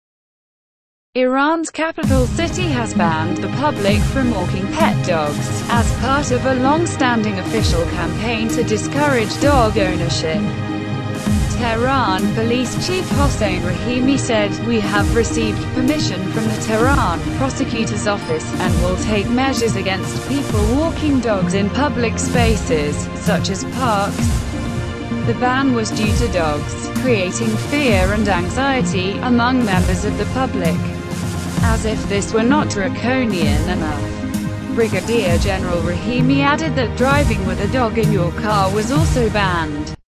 Tehran bans dog walking in public spaces
Iran's capital city has banned the public from walking pet dogs, as part of a long-standing official campaign to discourage dog-ownership.

Tehran Police Chief Hossein Rahimi said "we have received permission from the Tehran Prosecutor's Office, and will take measures against people walking dogs in public spaces, such as parks".